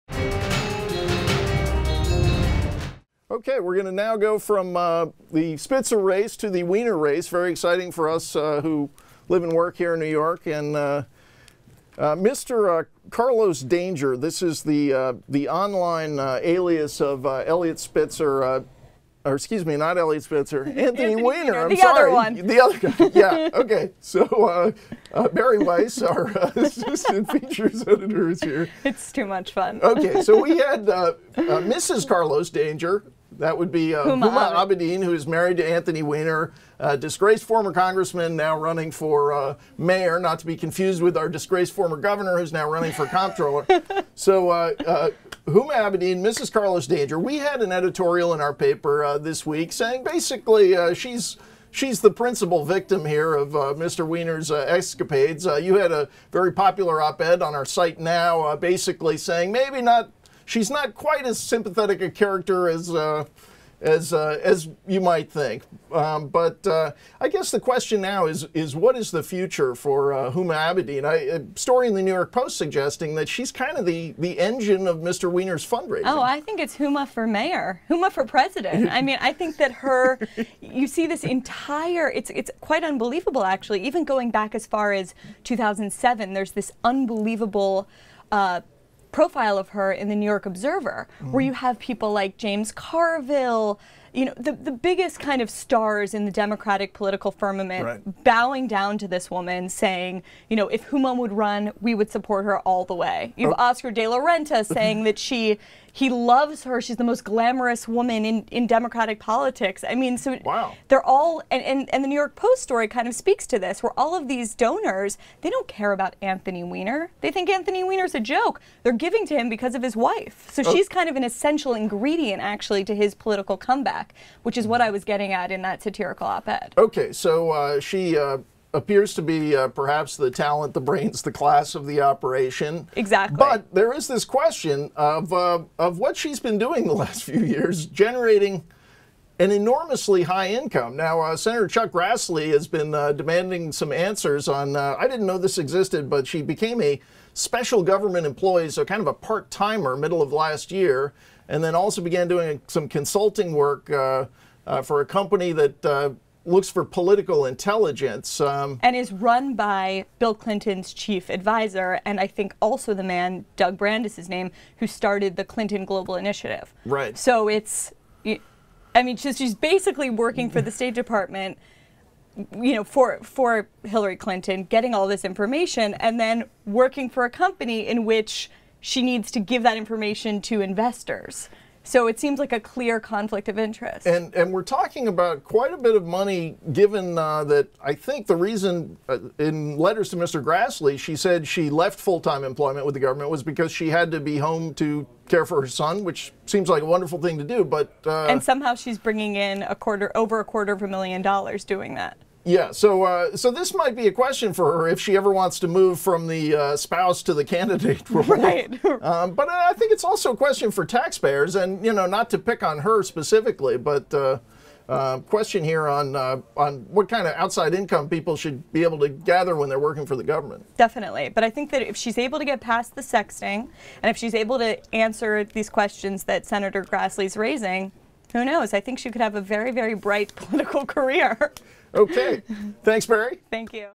Anthony Weiner's Wife, Huma Abedin: Mrs. Carlos Danger | WSJ Opinion
Assistant features editor Bari Weiss on why Huma Abedin is standing by Anthony Weiner. Photo: Associated Press